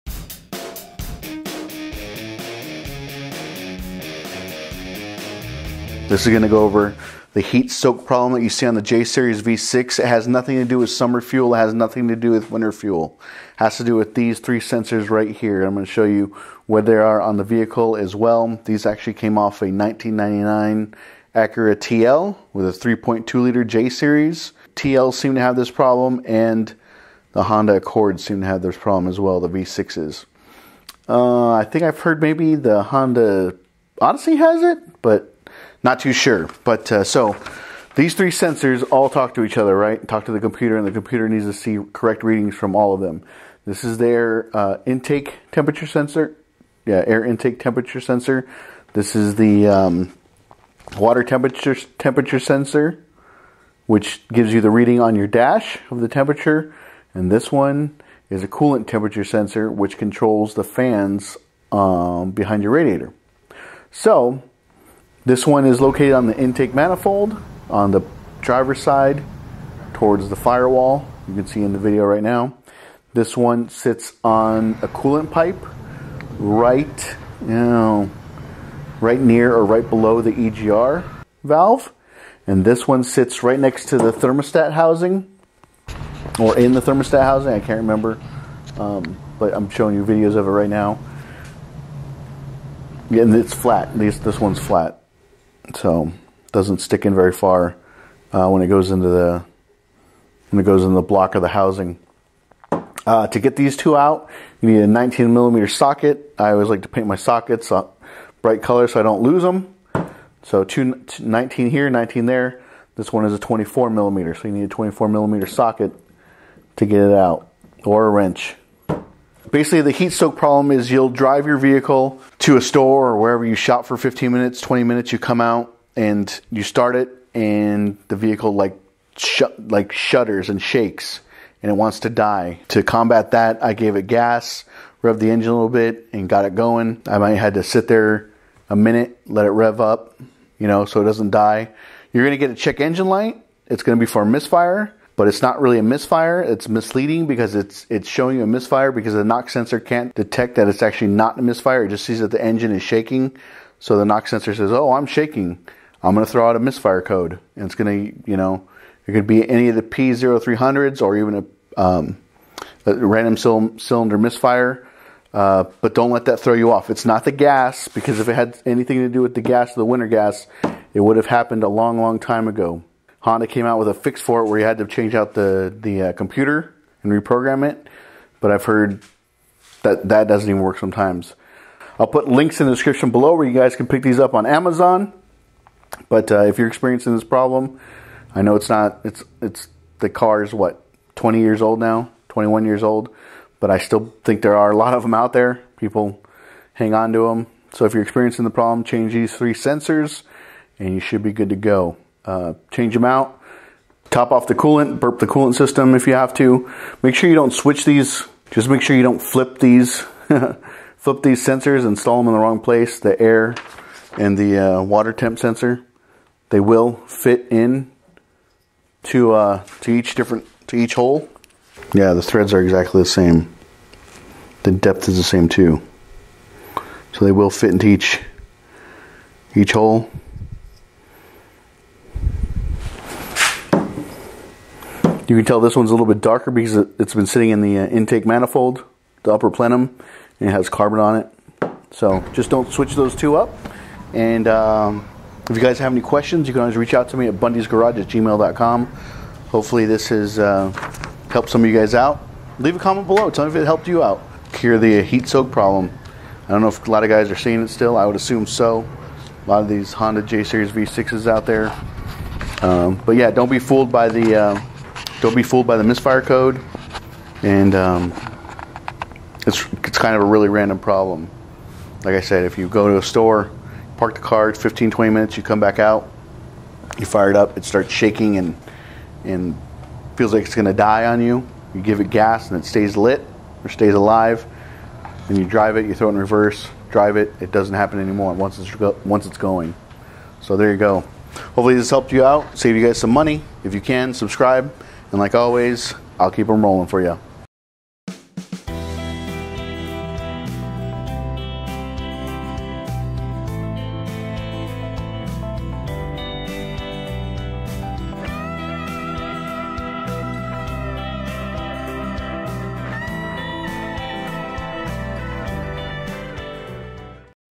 SOLVED V6 Honda Accord Vapor Lock Heat Soak Fix | J Series Acura TL | Bundys Garage
The Honda Acura heat soak problem that shows it's ugly head in the J Series V6 has been solved. It has nothing to do with summer fuel or winter fuel. It has to do with 3 sensors that go bad and need to be replaced. You don't need to replace the PCM and re-program it. Watch the video to see how to fix the problem.

AIR TEMP SENSOR

ENGINE COOLANT TEMP SENSOR

THERMOSTAT HOUSING TEMP SENSOR

Bundys Garage